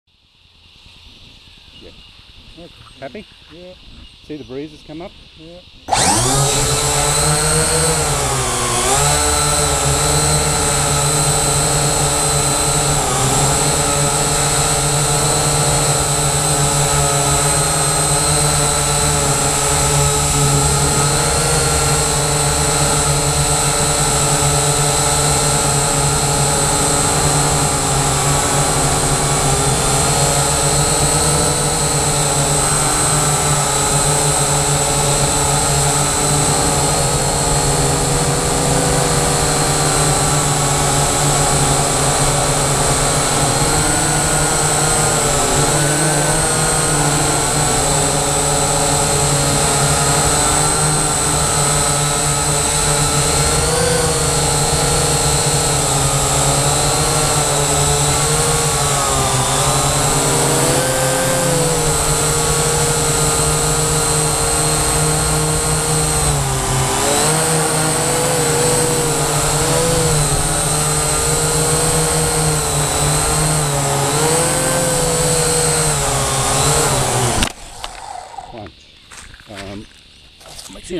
Syma X8C GOPR0 - one minute mission.
This was a video re-con over a patch of bush where a syma X5W was lost. We weren't optimistic but it was a fun thing to try.
I was planning a slightly longer flight but the gopro wifi downlink failed I so I terminated the flight. We did not find the black quadcopter.
I did a second flight in timelaspe still mode which showed more detail than the 1080p video. We won't be buying black drones any more.